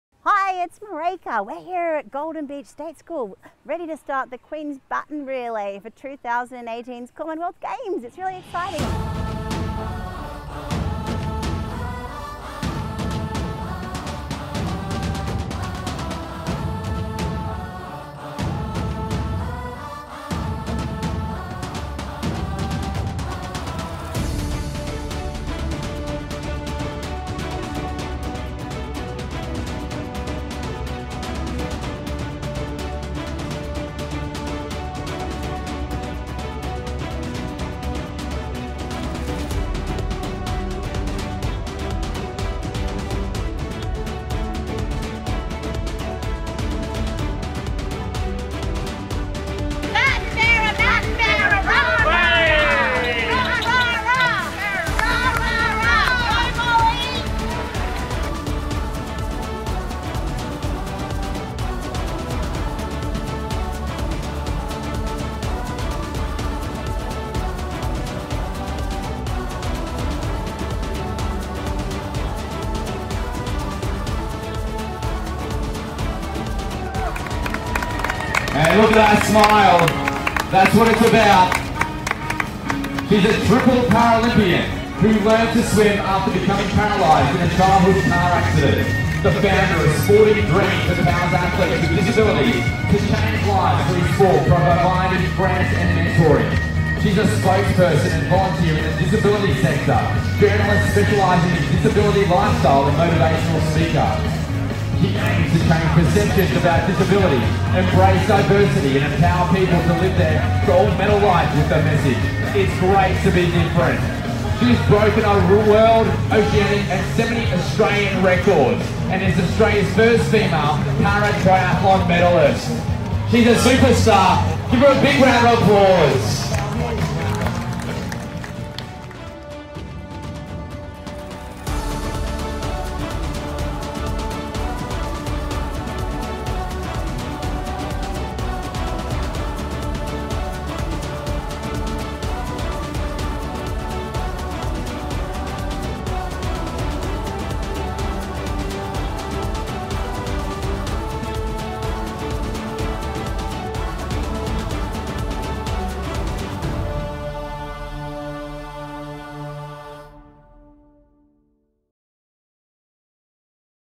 Marayke's Queens Baton Relay run
Marayke Jonkers, 33, is a retired Paralympic medal winning swimmer, Australia's first female paratriathlete to medal at a world championships and founder of Sporting Dreams, a grants program which helps athletes with disabilities. A former world record holder she competed at the Sydney 2000, Athens 2004 and Beijing 2008 Games winning 2 bronze and a silver medal. She also has two university degrees, and is a sought after thought leader and speaker in the field of self leadership empowering audiences to live their own gold medal life. She's been a finalist in the Young Australian of the Year and Young Queenslander of the Year and won the Cosmopolitan Magazine Fun fearless Female Award. for more